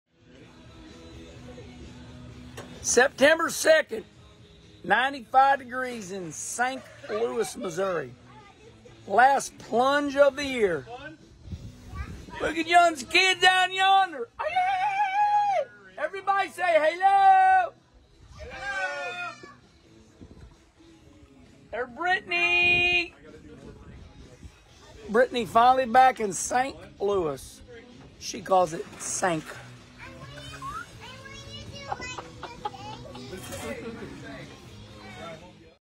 Last Pool Day of the Year?!
Kenny Wallace and family go for a swim.